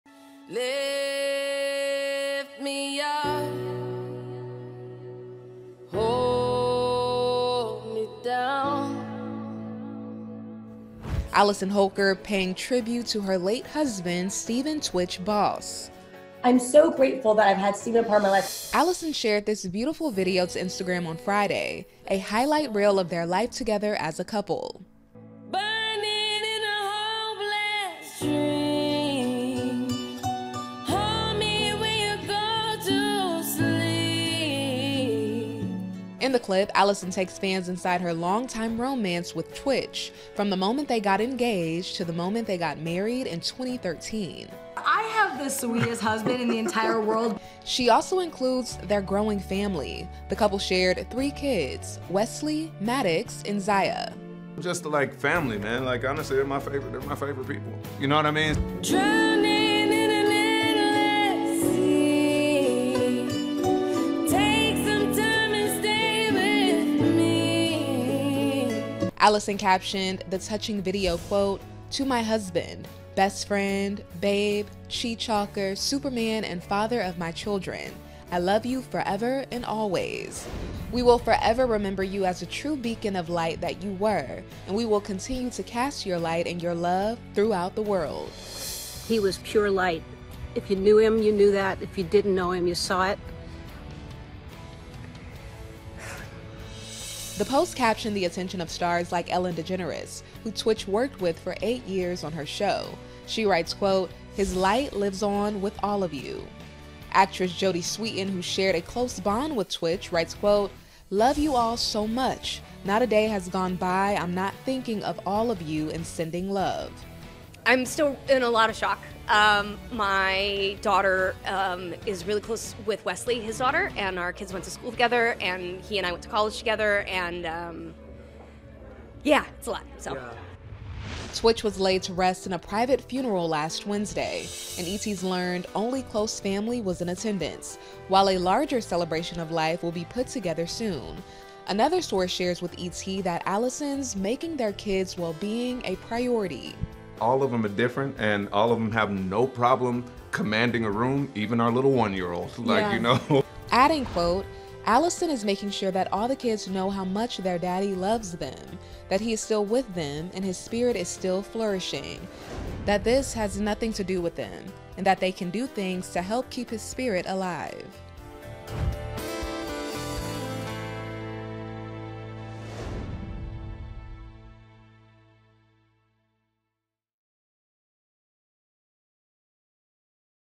Allison Holker Shares Love Story With Stephen 'tWitch' Boss in Sentimental Video
Allison Holker pays tribute to her late husband, Stephen 'tWitch' Boss, with a highlight reel of their love story on Instagram. This comes after ‘tWitch’ was laid to rest last Wednesday, nearly a month after his death. Allison and ‘tWitch’ shared two children, Maddox, 6, and Zaia, 3. Stephen’s also dad to Allison’s 14-year-old daughter, Weslie, from a previous relationship.